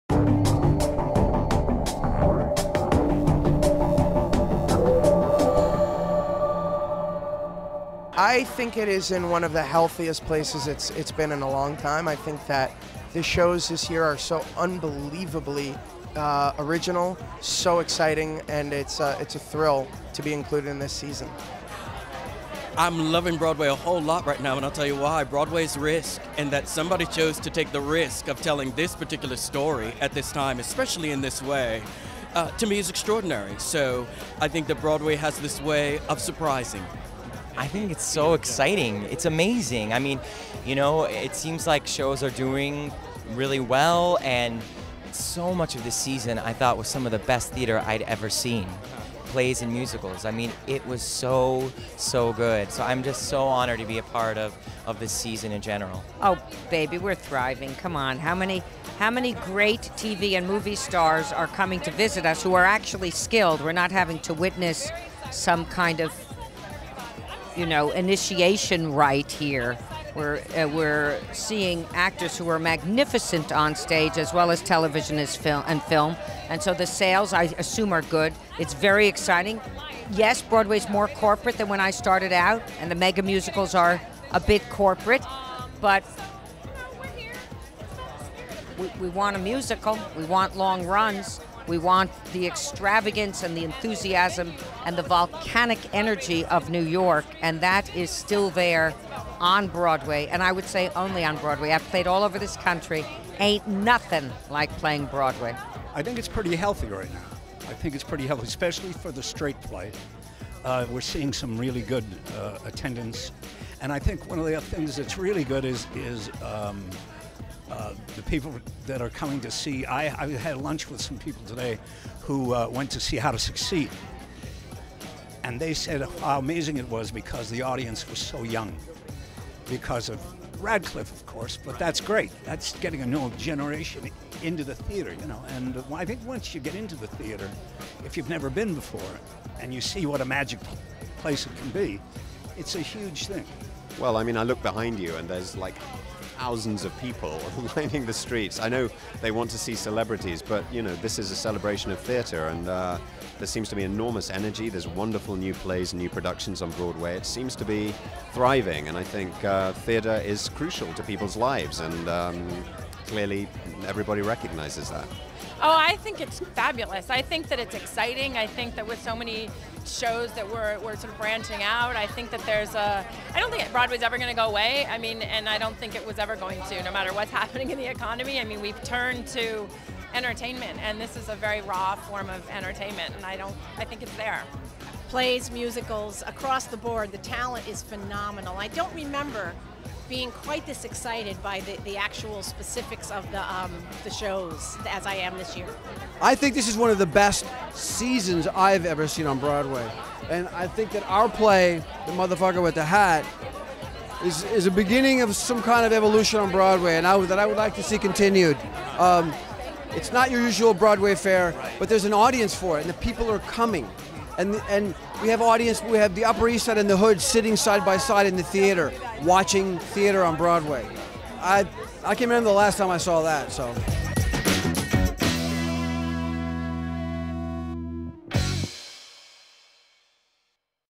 2011 Tony Nominees on the State of Broadway Theater
Nominees, winnners, and presenters at the 65th annual Tony Awards talk about the current state of Broadway. Including Josh Gad and Rory O'Malley ('The Book of Mormon'), Adam Godley ('Anything Goes'), Yul Vazquez ('The Motherf***er with the Hat'), Tovah Feldshuh, Len Cariou, Brooke Shields, Edie Falco, and more.